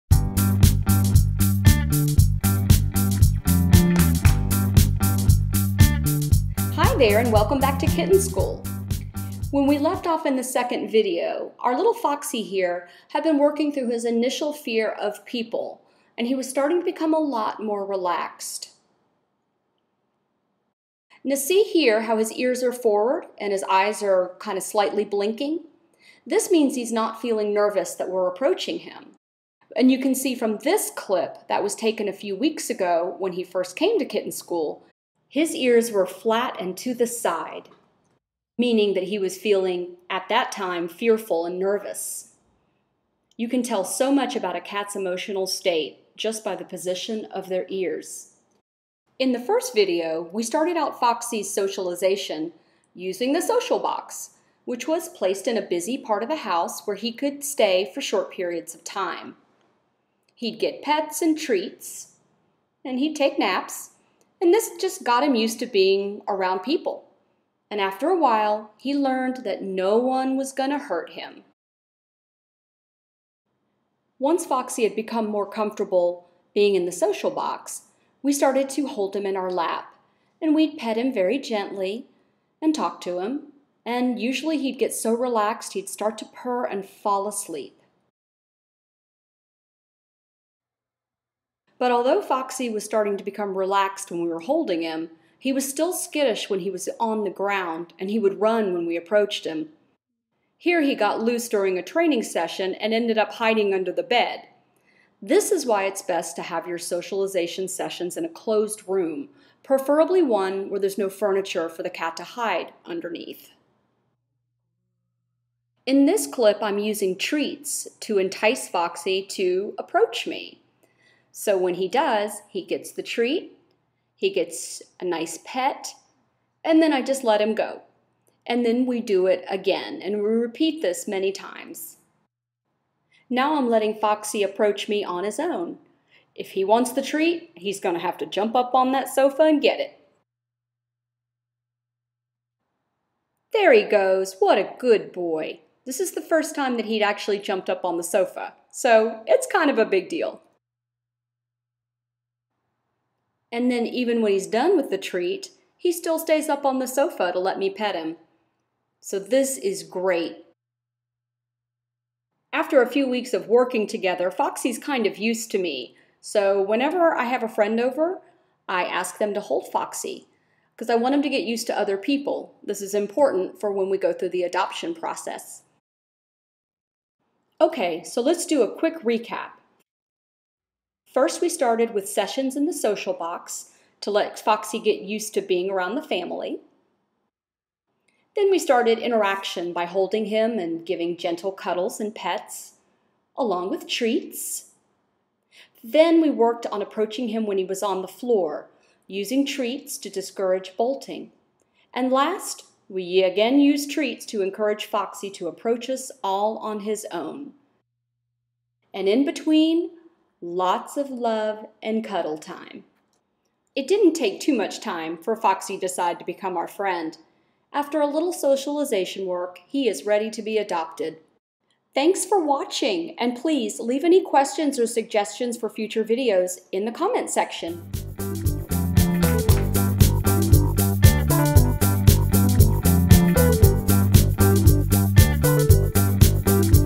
How to Make friends with a Scared, Shy Kitten - Foxy - Part 3 of 3
Little Foxy has come a long way since he came to Kitten School as a frightened, cringing little guy who had never been held by a human. We have worked hard to gain his trust and friendship, and now he's ready for his Forever Home! This is the last video in a 3 part series showing how to tame a shy or frightened cat or kitten.